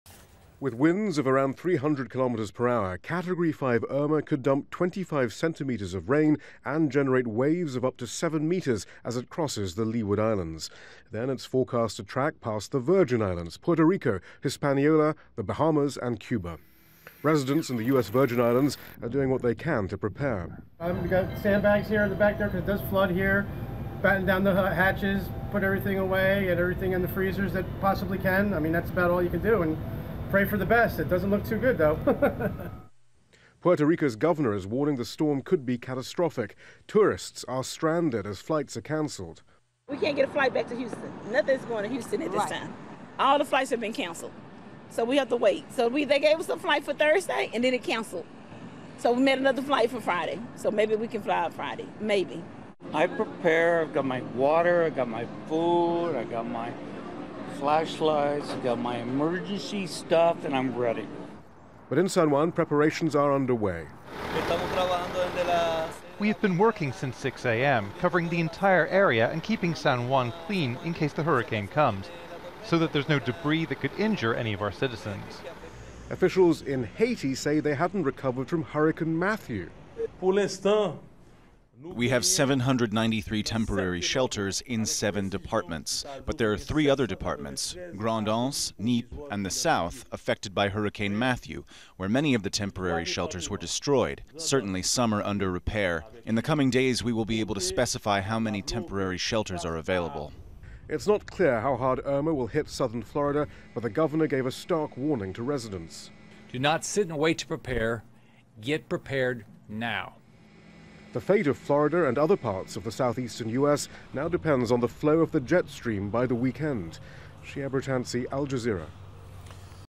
Hurricane Irma threatens Caribbean islands and Florida
Hurricane Irma has strengthened into what is being described as a “potentially catastrophic” Category 5 storm as it moves across the Atlantic Ocean.

The southern Bahamas has started the biggest evacuation in its history. And an emergency has been declared in Puerto Rico, the US Virgin Islands and the US state of Florida.

Al Jazeera's Shihab Rattansi reports.